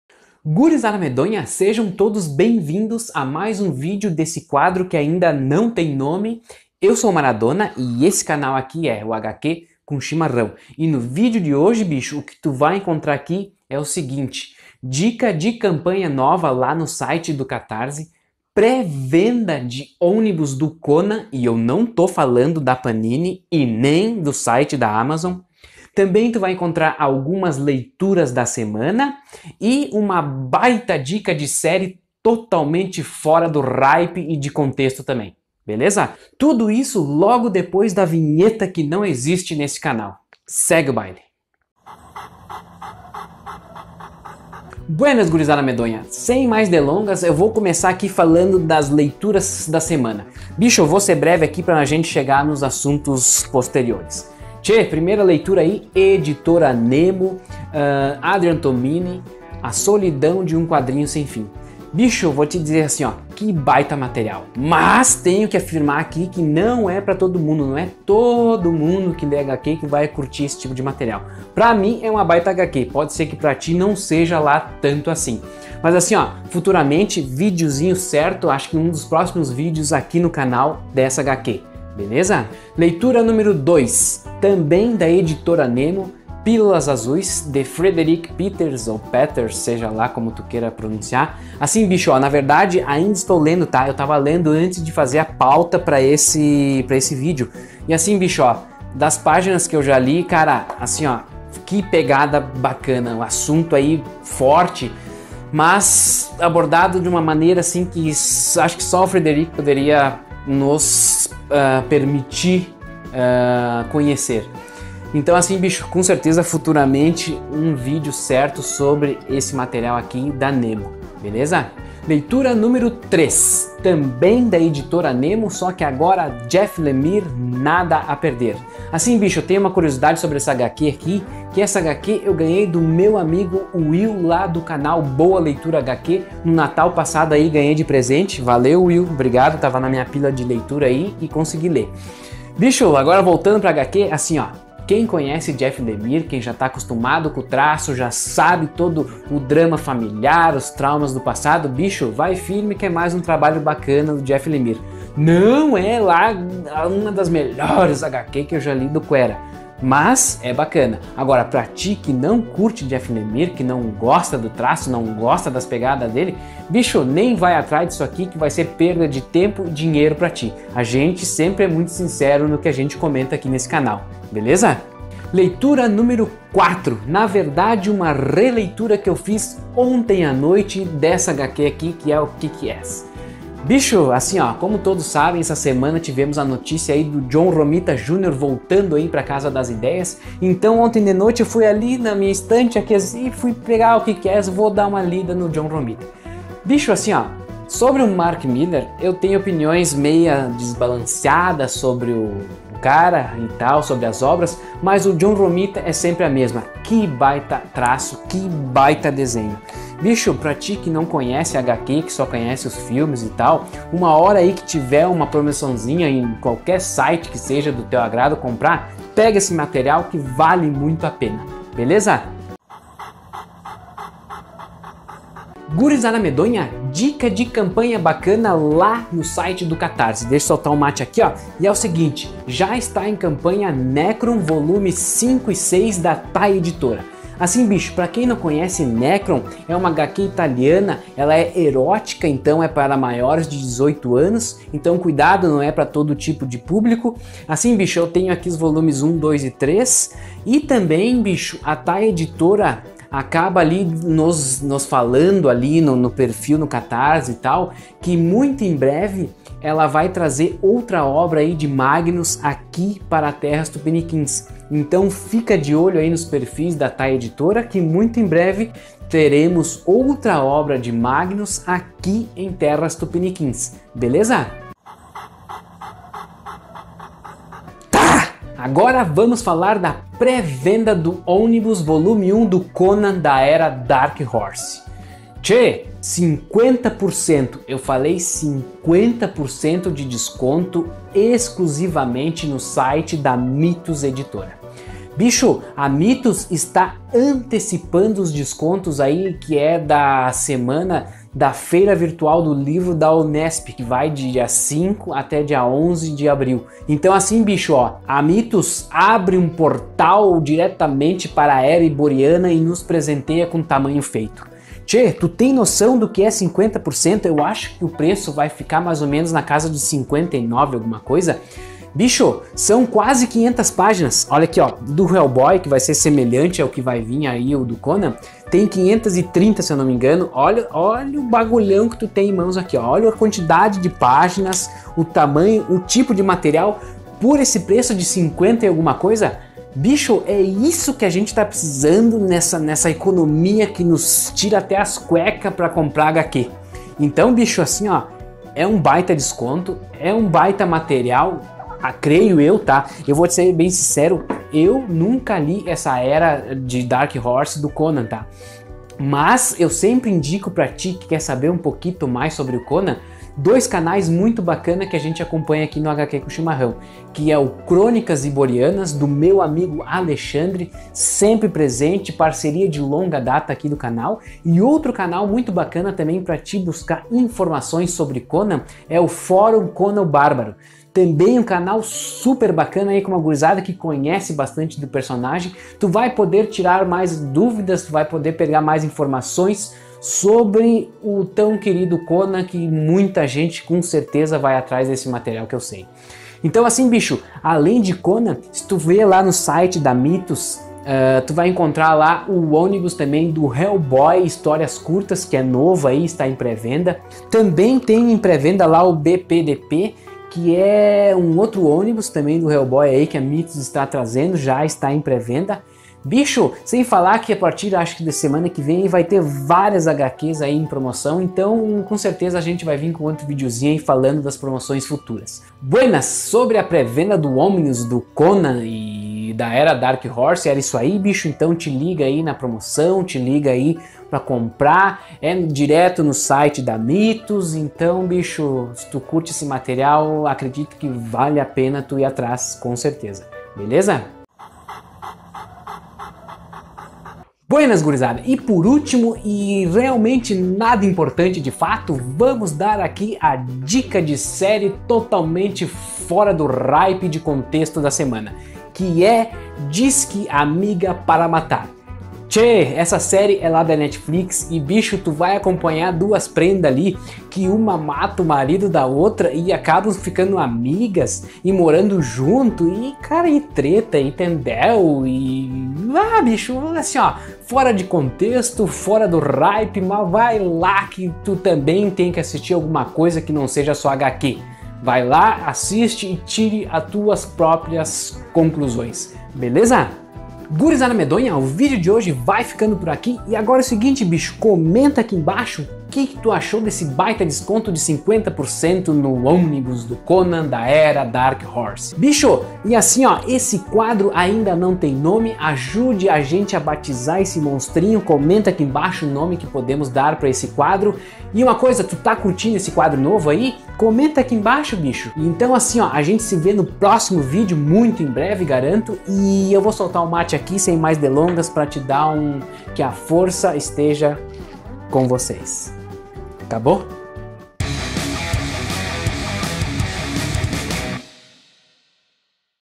PRÉ-VENDA OMNIBUS CONAN MYTHOS EDITORA, NECRON 5 E 6 NO CATARSE, LEITURAS DA SEMANA E DICA DE SÉRIE
PRÉ-VENDA OMNIBUS CONAN MYTHOS EDITORA, NECRON VOL. 5 E 6 TAI EDITORA EM CAMPANHA NO CATARSE, LEITURAS DA SEMANA, DICA DE SÉRIE E MUITO CHIMARRÃO!